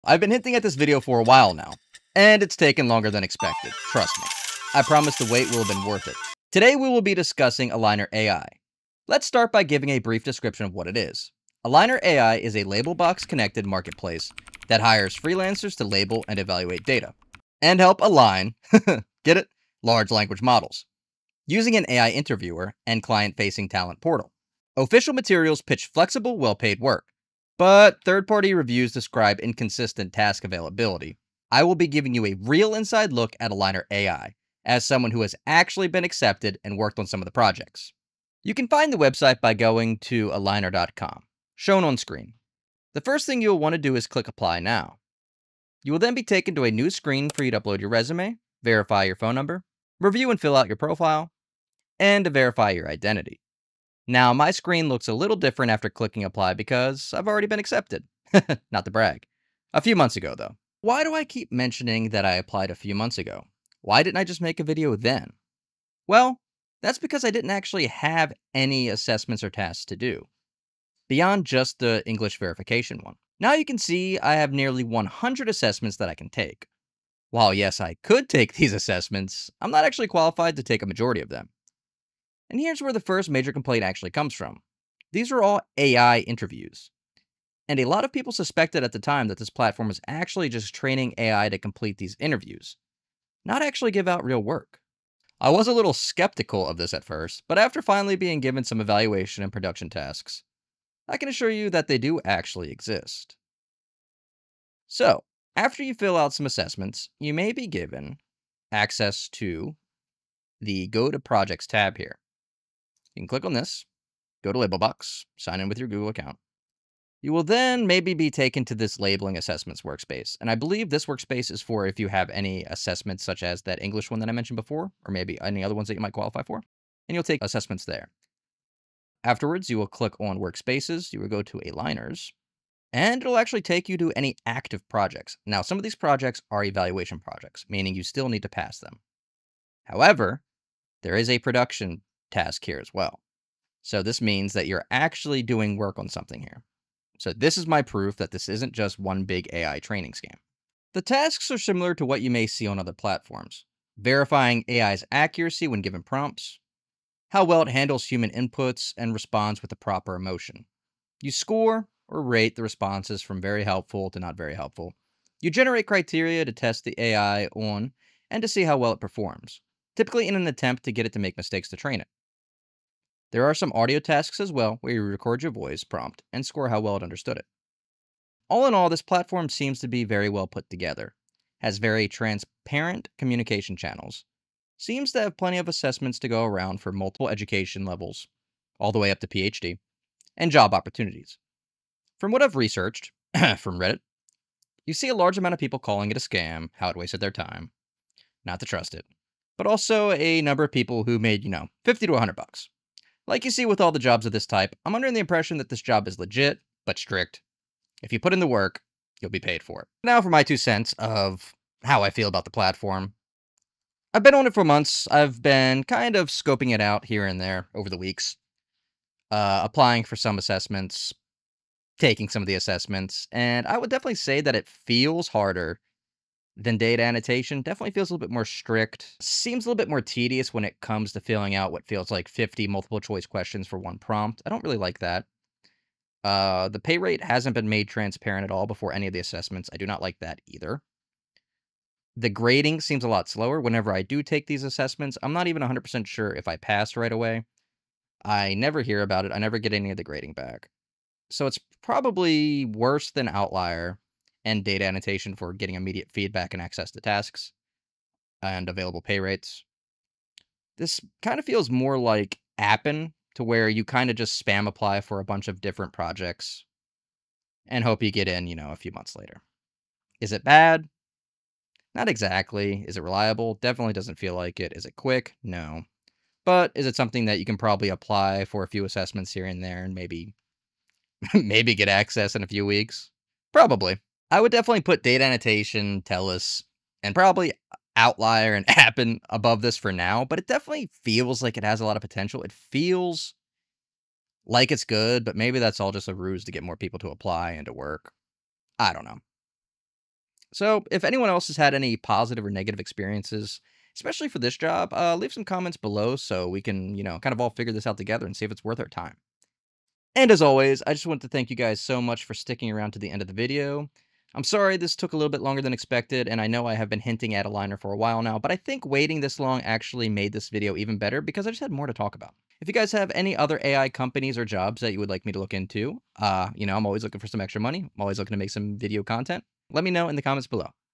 The Worst AI Platform yet (Alignerr AI) - My experience so far as a rater.
Today we will be discussing Alignerr AI, let's start by giving a brief description of what it is, "Alignerr AI is a Labelbox-connected marketplace that hires freelancers to label and evaluate data and help “align” ha i get it. LLMs, using an AI interviewer (get used to this) and client-facing talent portal. Official materials pitch flexible, well-paid work, but third-party reviews describe inconsistent task availability"  and that brings us todays video I will be giving you a real inside look at aligner AI as someone who has actually been accepted, and worked on some projects.

Images and video sourced via Vecteezy consider using my affilate link here "